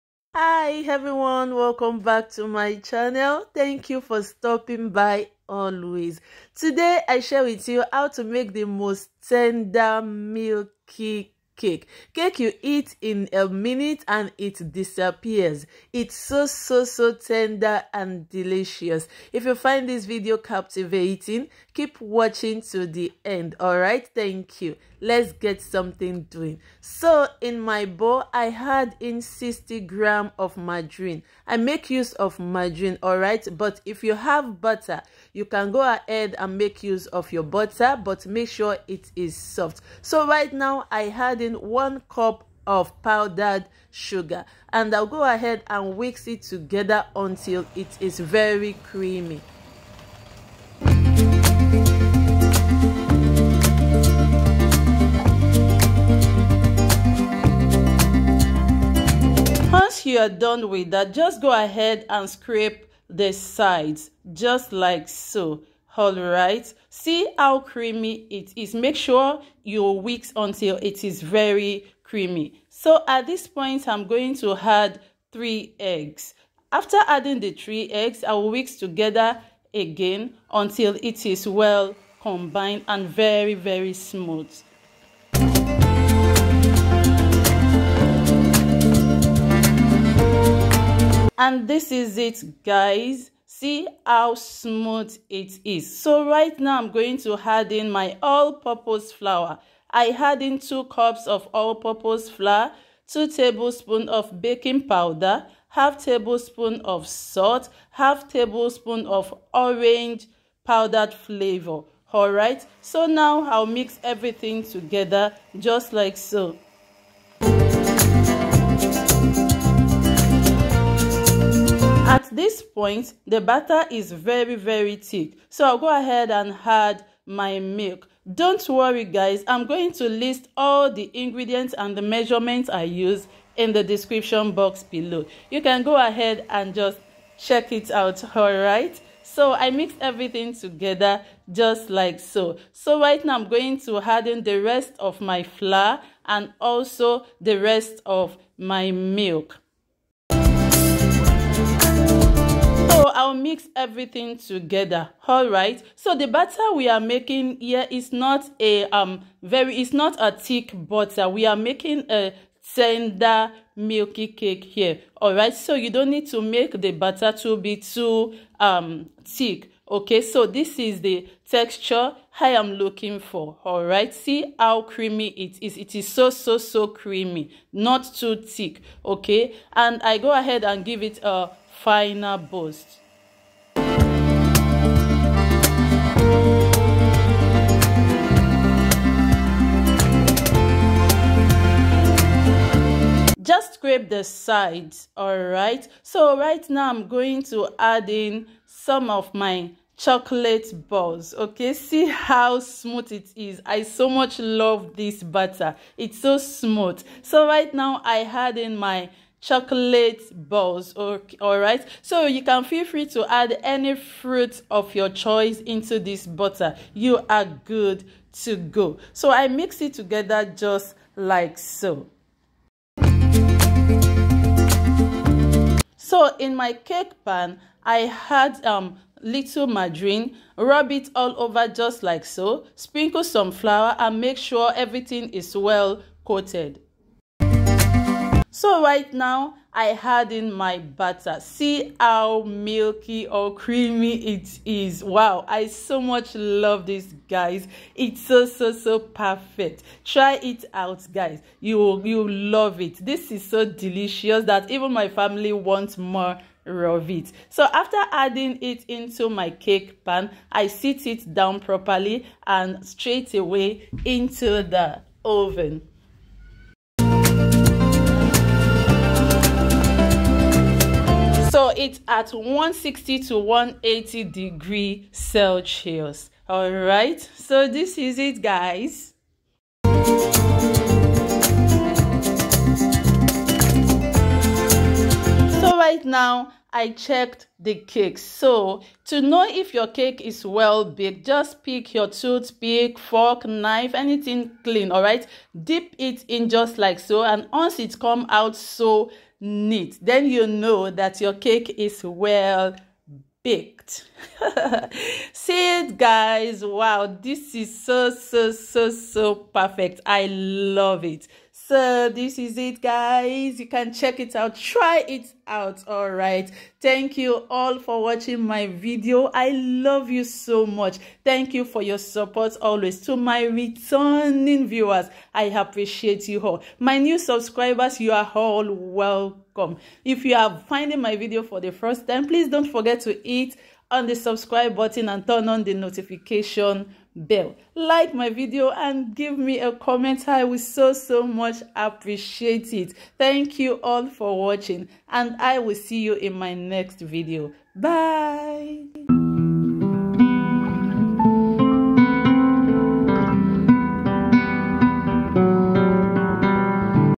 The most Amazing melting cake recipe | How to make milk cake.
Here are the list of ingredients and measurements need for this awesome tender milk cake 🎂

Ingredients:
60gm magerine
1cup of powdered sugar
3 eggs 🥚
3 cup of all purpose flower
2tsp of baking powder
1/2 tea spoon of salt
1/2tsp of orange 🍊 power flavour
2 cups of milk 🥛
Some chocolate 🍫 balls